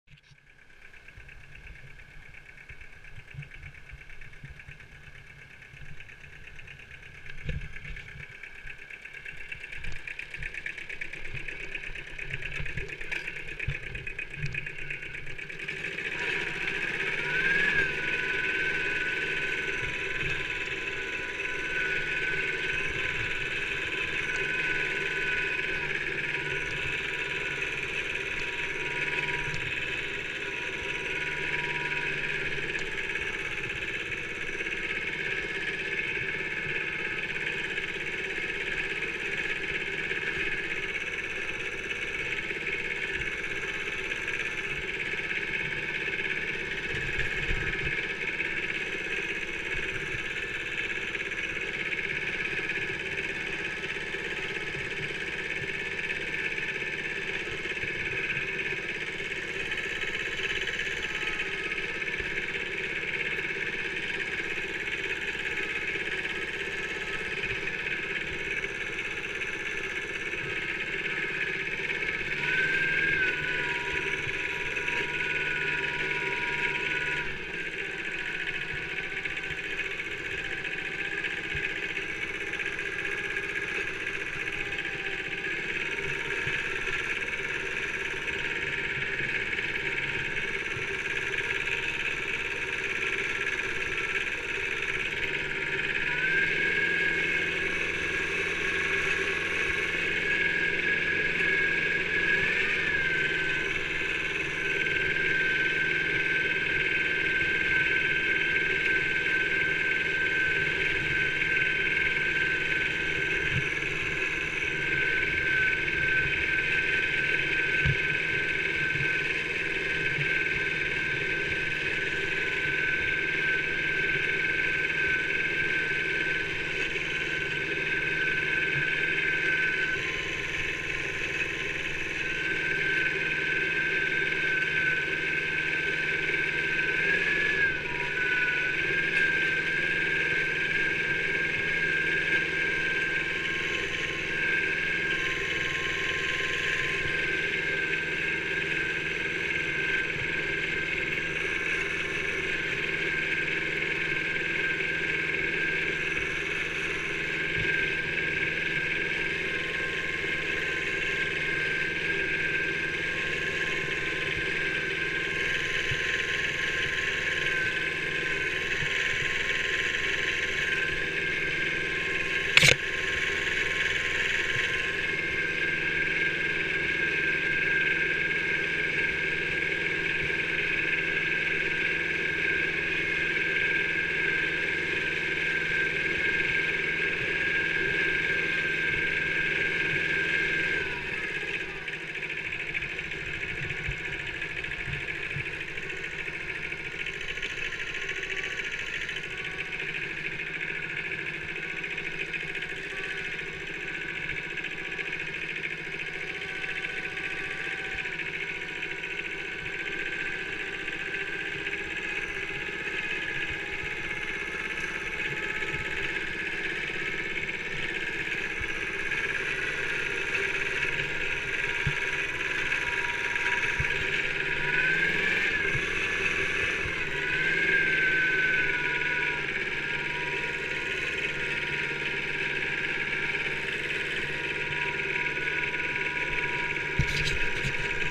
Kubota B7000, forest driving at winter
The Kubota B7000, the legendary minitractor at winter driving. Ice grip chains in the front and rear tires. The atmosphere is maximized! Only Kubota can do this!